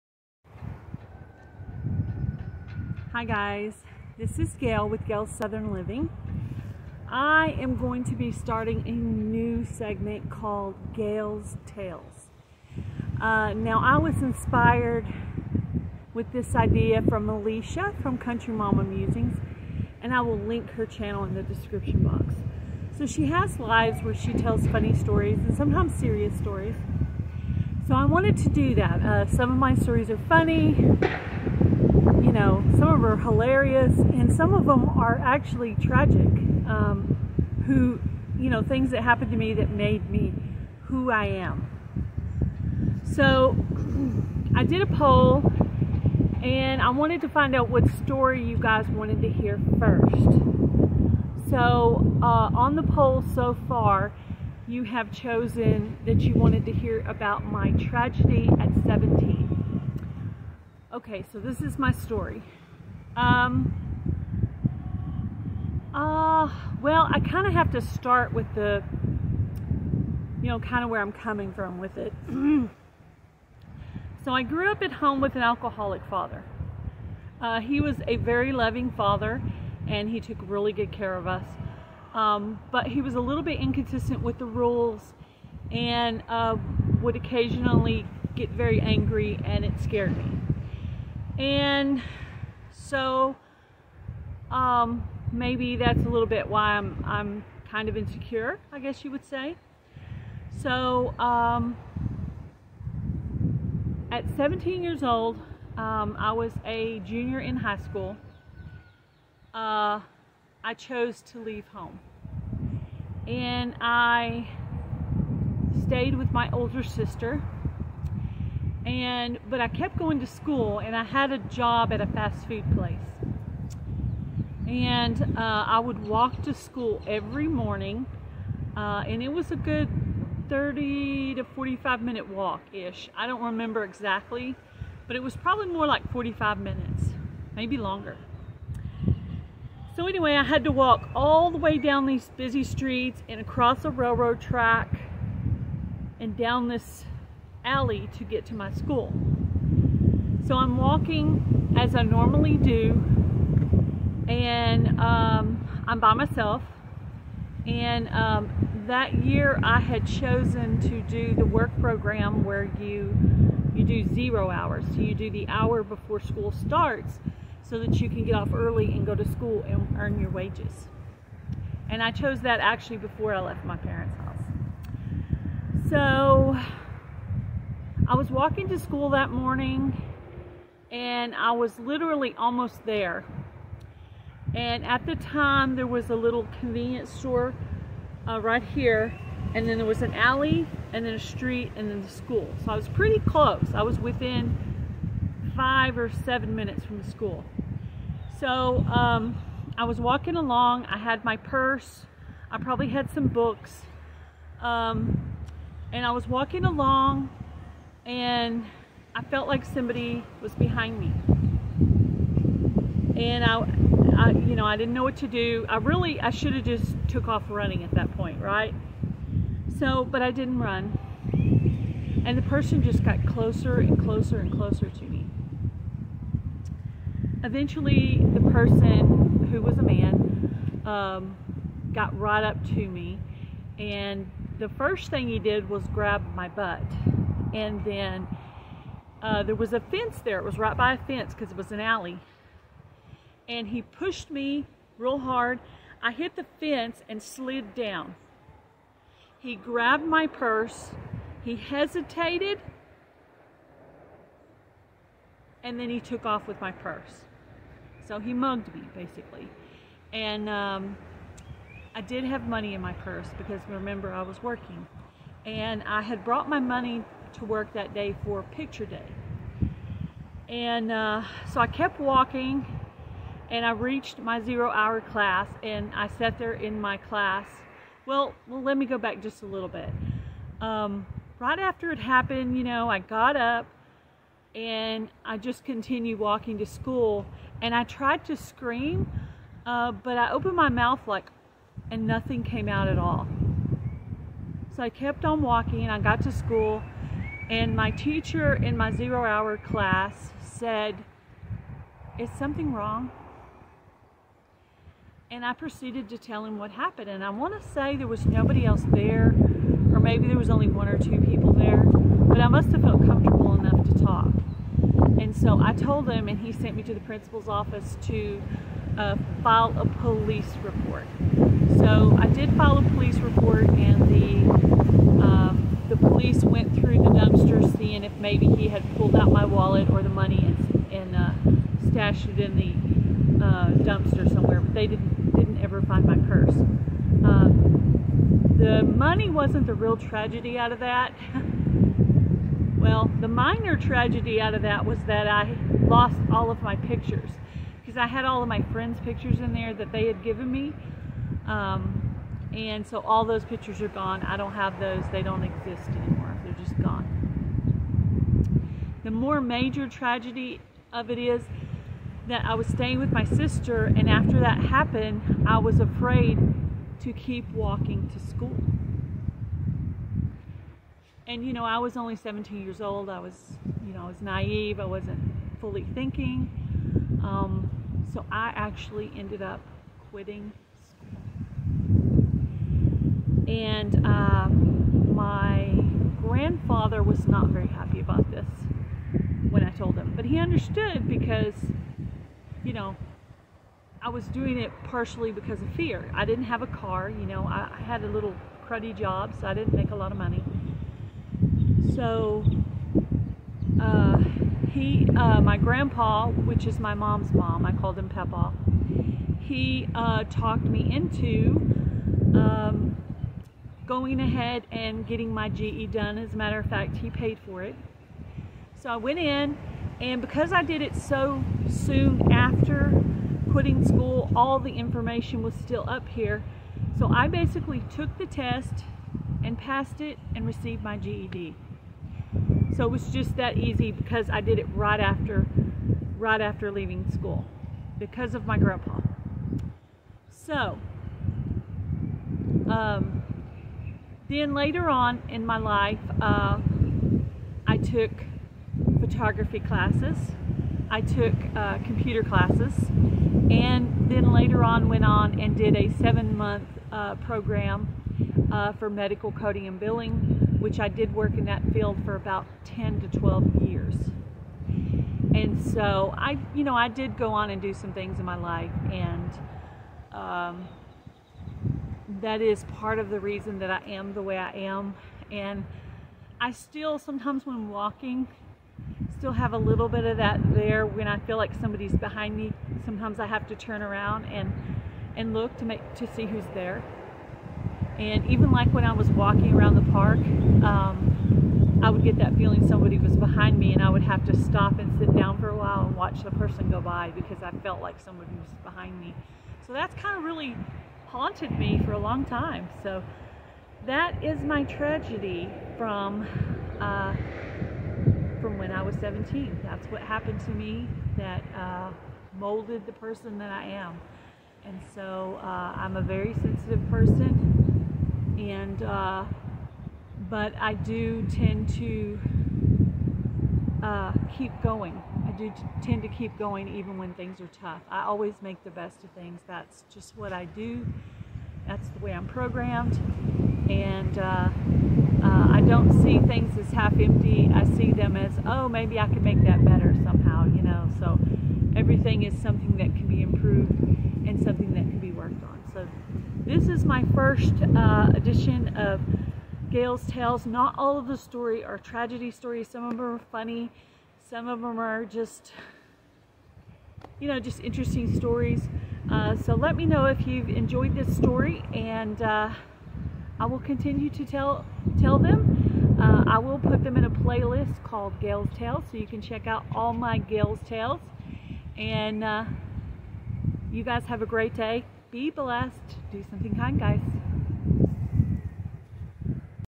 Gail's Tales 1 - My Tragedy At 17 Years Old
A first Addition to my series named Gail's Tales.  My Tragedy at 17 year old and how it shaped me.

GO CHECK OUT HER CHANNEL
Country Mama Musings